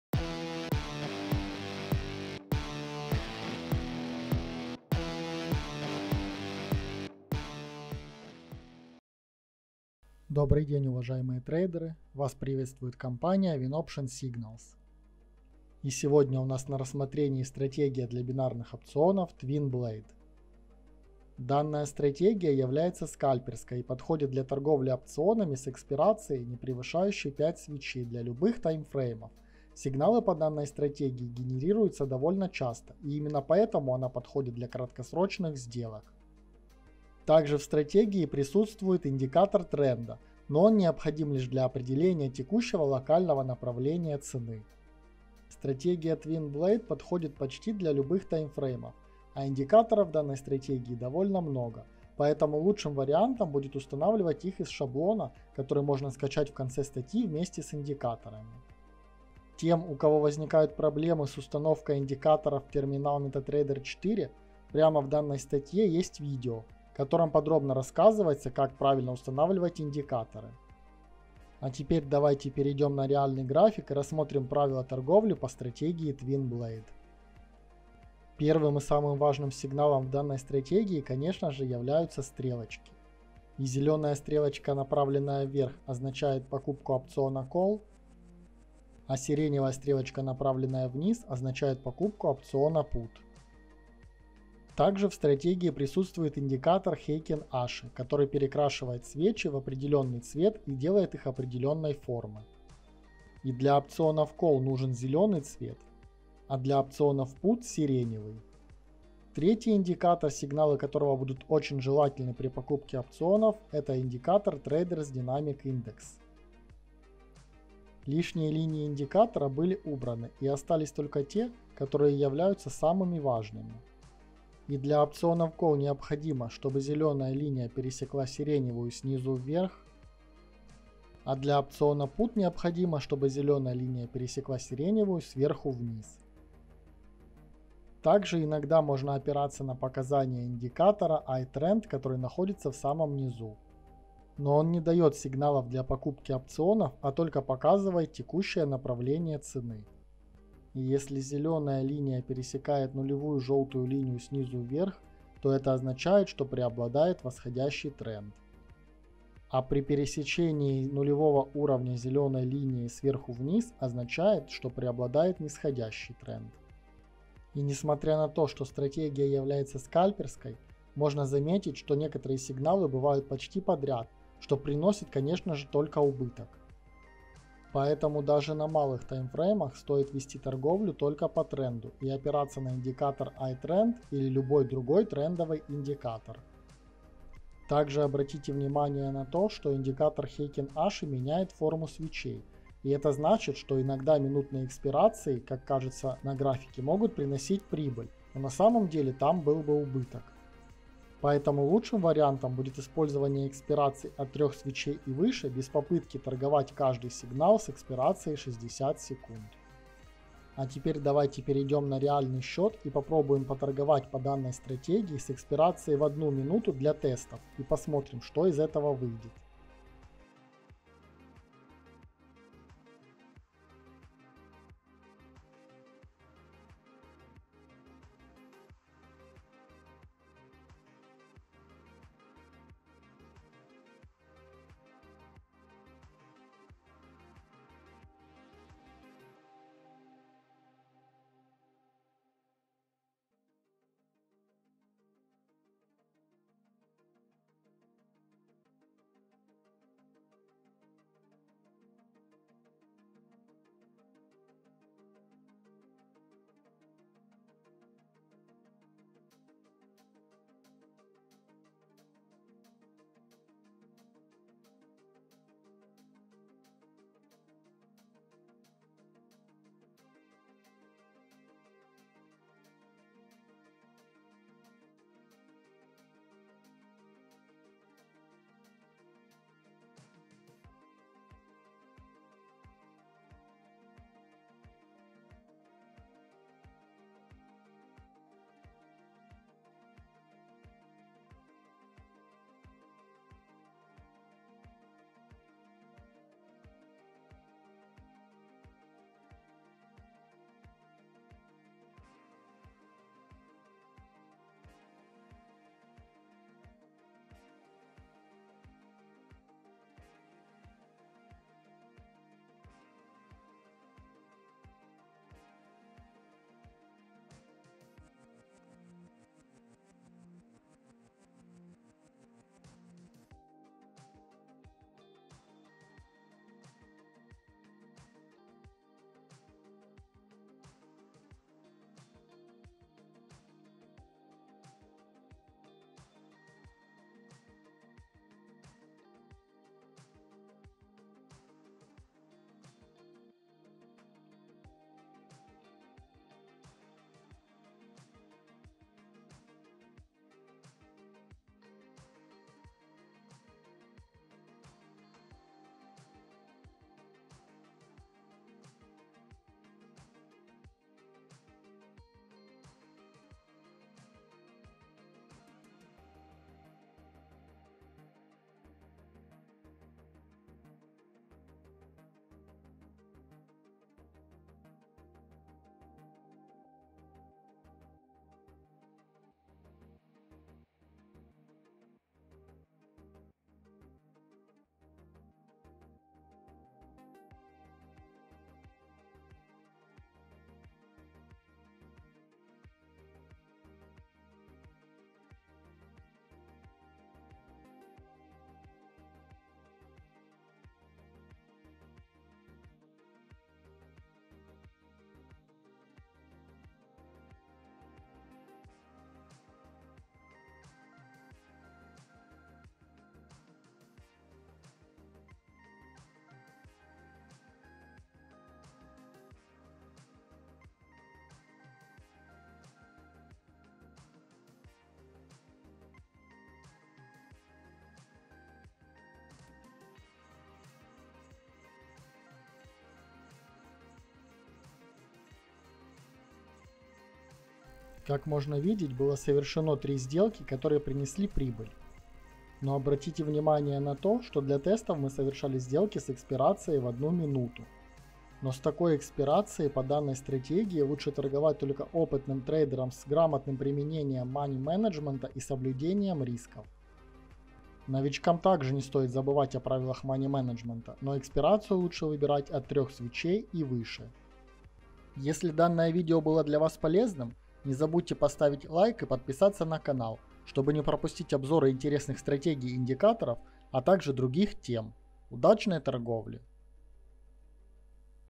Стратегия Twin Blade для бинарных опционов. Скальпинг. Binary Option Strategy
В этом видео мы расскажем о такой стратегии для бинарных опционов, как Twin Blade. Данная стратегия основана на нескольких индикаторах, включая один сигнальный и один трендовый, а также индикатор Heiken Ashi. Торговля по этой стратегии для бинарных опционов позволяет совершать скальперские сделки как с экспирацией 60 секунд, так и с экспирацией 5 минут. Сделки по данной стратегии мы совершали на торговой платформе брокера Покет Оптион, где смогли получить прибыль.